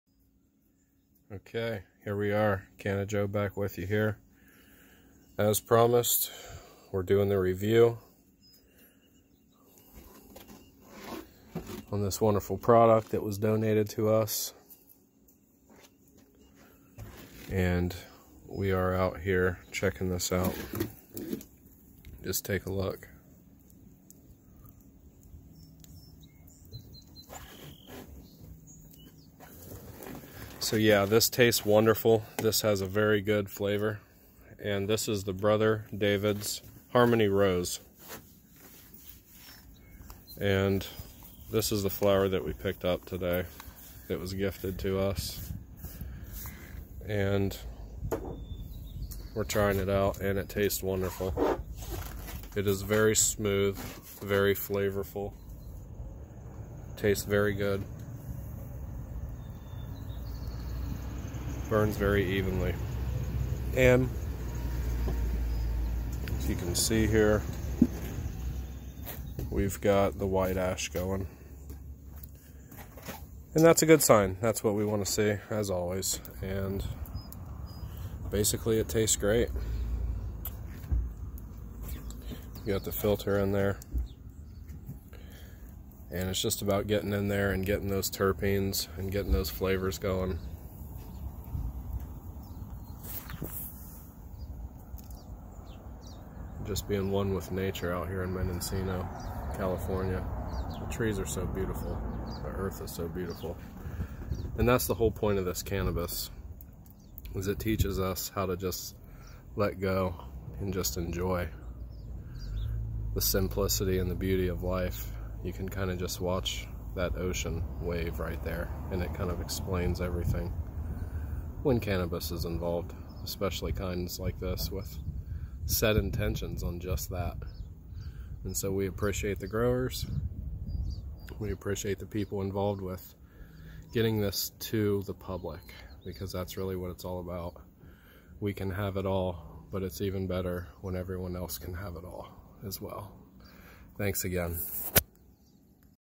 Brother David's Harmony Rose 1:1 Flower Review
Hang out with CannaJoe in Mendocino and experience one of the best flowers yet. You can really tell the intention set by The Growers and people involved with this delightful product.